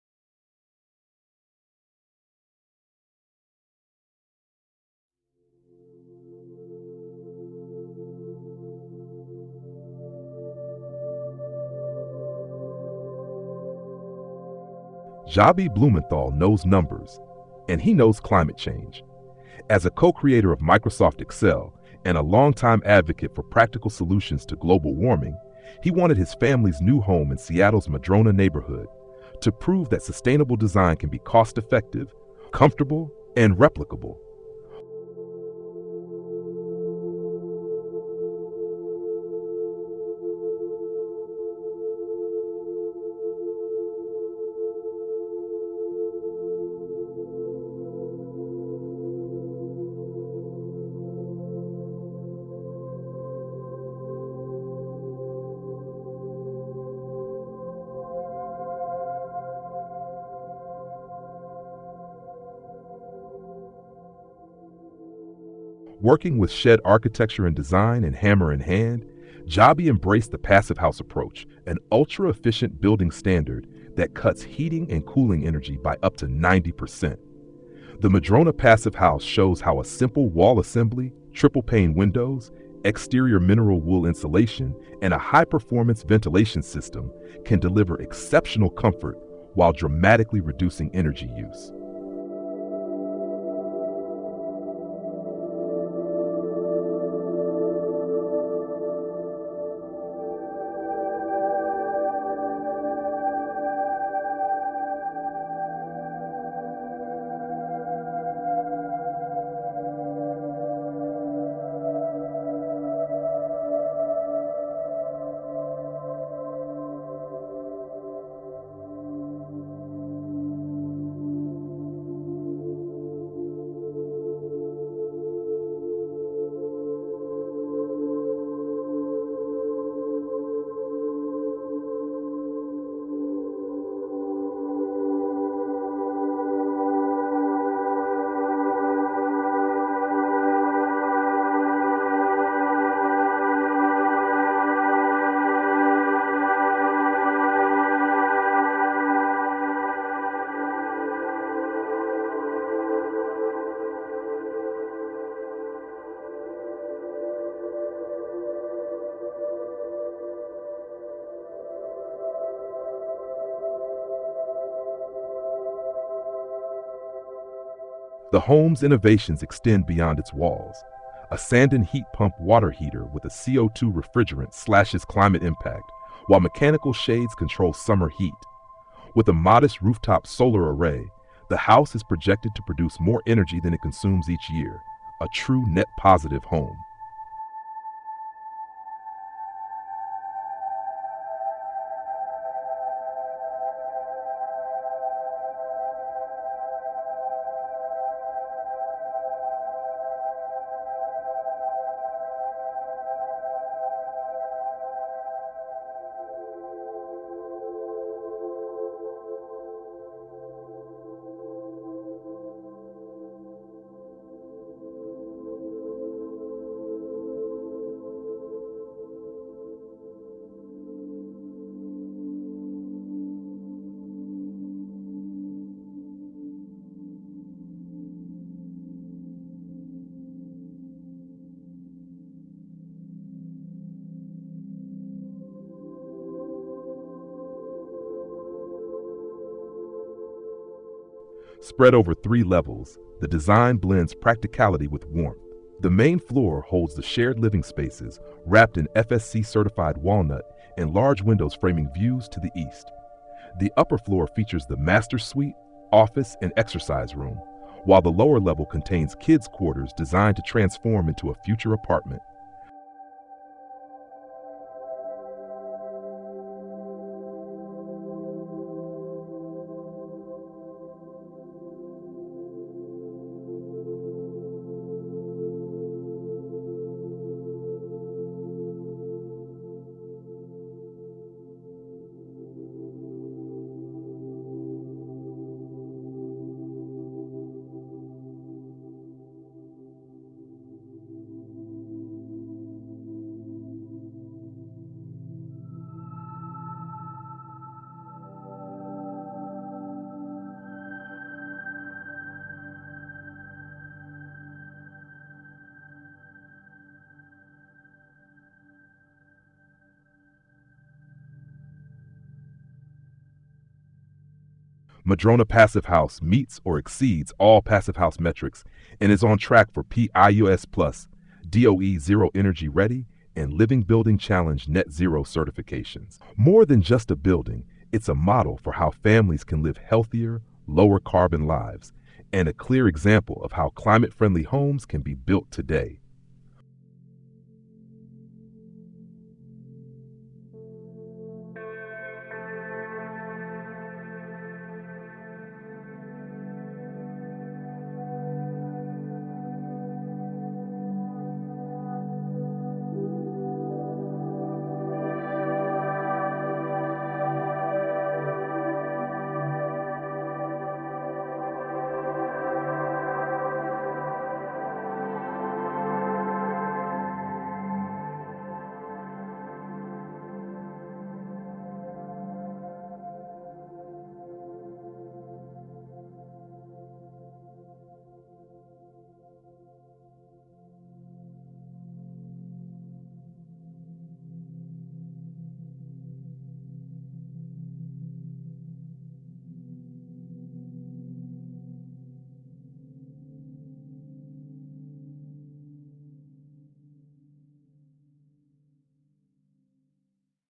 Madrona Passive House – Ultra-Efficient Home Powered by the Sun
Meet Madrona Passive House – a net-positive energy home in Seattle designed for Jabe Blumenthal (co-creator of Microsoft Excel) and his family. This cutting-edge yet cost-conscious project shows how high-performance, climate-friendly building can become mainstream.

✨ Key Highlights:
- Passive House Design: Reduces heating and cooling energy by up to 90% using a simple, replicable wall assembly.
- High-Performance Envelope: FSC-certified cedar siding, mineral wool “winter coat,” triple-pane Zola windows, and a Zehnder HRV for superior indoor air quality.
- Pioneering Systems: CO₂-based Sanden heat pump water heater, mechanical exterior shades, and a 9.8kW rooftop solar array designed to generate more power than the home consumes.
- Flexible Layout: Three levels linked by an open stair; lower level prepped to become an accessory dwelling unit.
- Beautiful + Sustainable: FSC walnut floors, built-in cabinetry with fun color accents, and concrete finishes that frame sweeping views of Seattle’s Madrona neighborhood.
- Certifications Expected: PHIUS+ Passive House, DOE Zero Energy Ready Home, and Living Building Challenge Net Zero Energy.

Why It Matters:
Madrona Passive House shows that elegant, energy-positive living doesn’t have to be experimental or prohibitively expensive. It’s a roadmap for making low-carbon homes mainstream.

Contractors: Hammer and Hand
Engineers: Todd Perbix
Photo: Mark Woods
Roxul: ROCKWOOL International A/S
Manufacturers: Zehnder
Windows: Zola Windows
Location: Seattle, WA, USA
Category: Private Houses